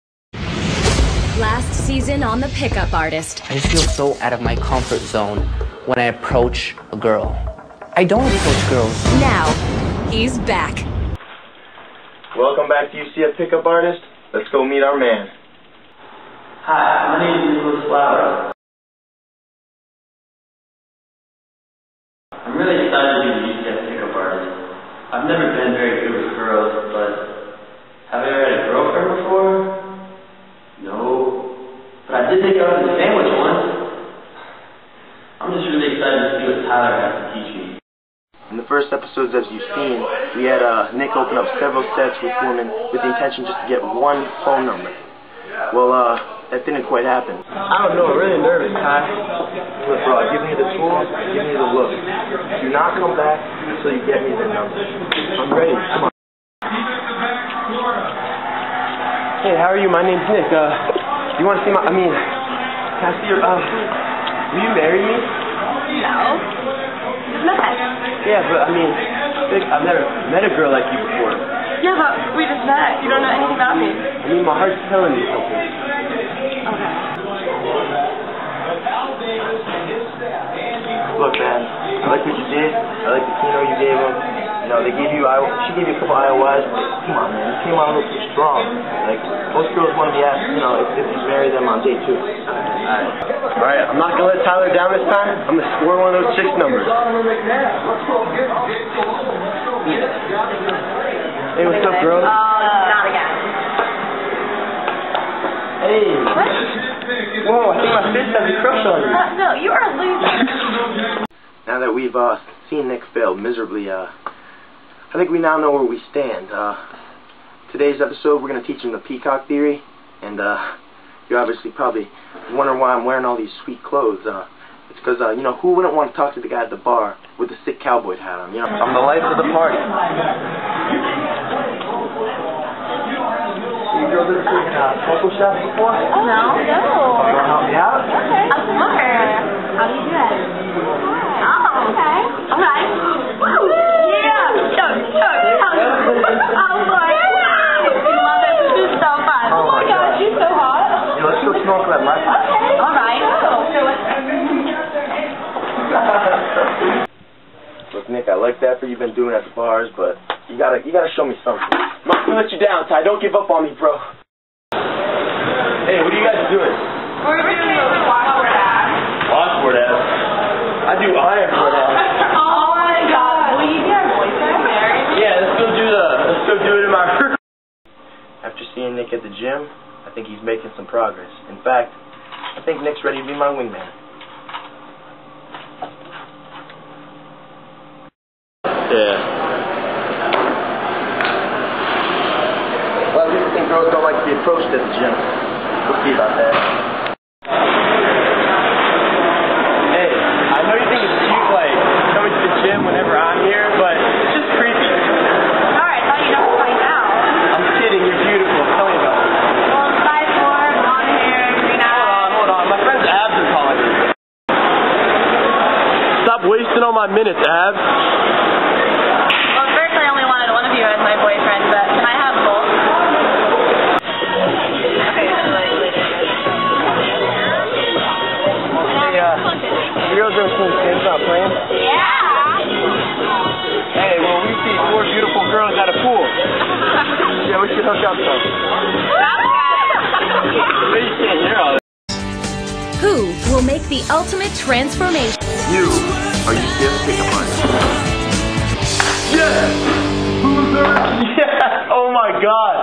How to GET GIRLS (Pickup Artist spoof)
basically mocking the show by mtv called the pickup artist where the "womanizer guru (master pickup artist) turns loser guys into studs by dressing them with crazy things called peacocking (basically flaring) and giving them the tools/lines and confidence to score girls at clubs. Ours is everything exaggerated times 10 .. basically going into clubs with a snorkel mask etc. Just see for yourself.